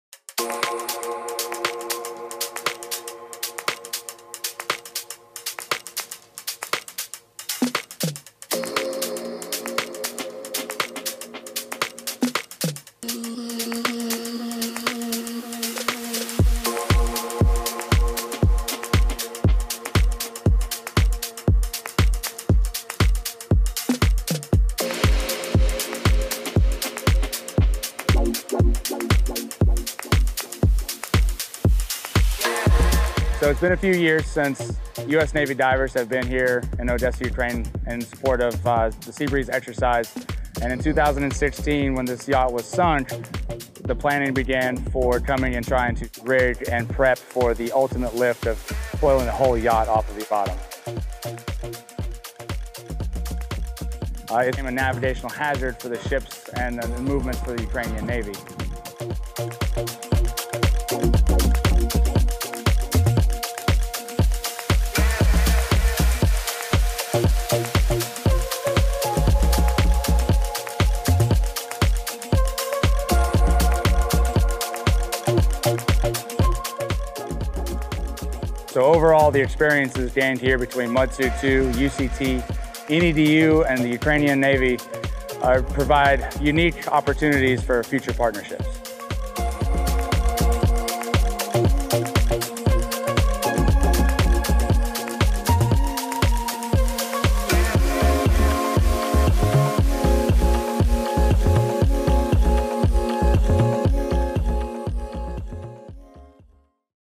U.S. and Ukrainian Navy Divers and Salvage during Exercise Sea Breeze 21
ODESA, Ukraine (July 9, 2021) U.S. and Ukrainian Navy Divers conduct dive and salvage operations together, working to clear a sunken vessel that has blocked the Port of Odesa for years during Exercise Sea Breeze in Odesa, Ukraine, July 7, 2021. Exercise Sea Breeze is a multinational maritime exercise co-hosted by U.S. Sixth Fleet and the Ukrainian Navy in the Black Sea since 1997. Sea Breeze 2021 is designed to enhance the interoperability of participating nations and strengthen maritime security and peace within the region. (U.S. Navy video by Mass Communication Specialist 2nd Class Cole Keller)